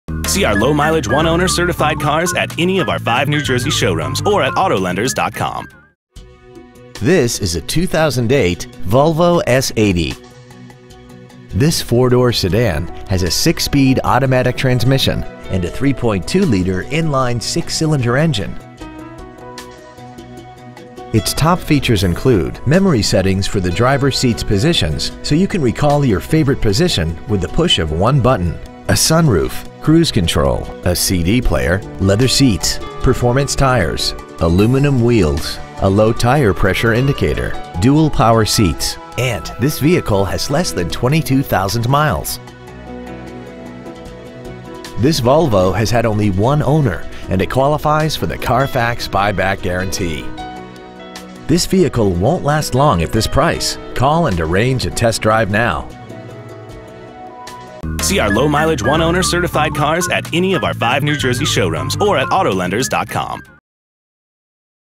2008 Volvo S80 Egg Harbor Township NJ
Year: 2008
 Make: Volvo
 Model: S80
 Engine: Gas I6 3.2L/195
 Trans.: Automatic
 Exterior: Barents Blue Metallic
 Miles: 21,484
 Interior: Sandstone Beige Leather
 Stock: V074555

 Auto Lenders